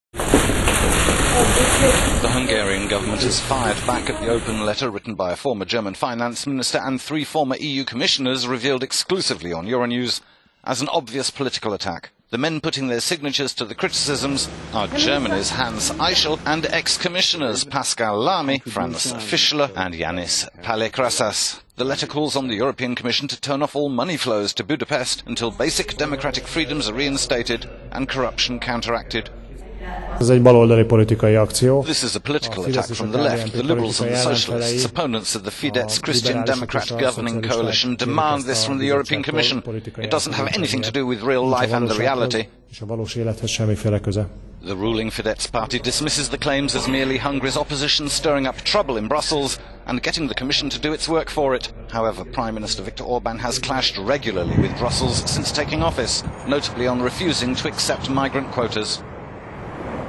Hungary says critical open letter an "obvious political attack"
The Hungarian government accuses the opposition of enrolling Brussels to resist the Fidesz-Christian Democrat coalition, and says criticisms bear no relation to reality.…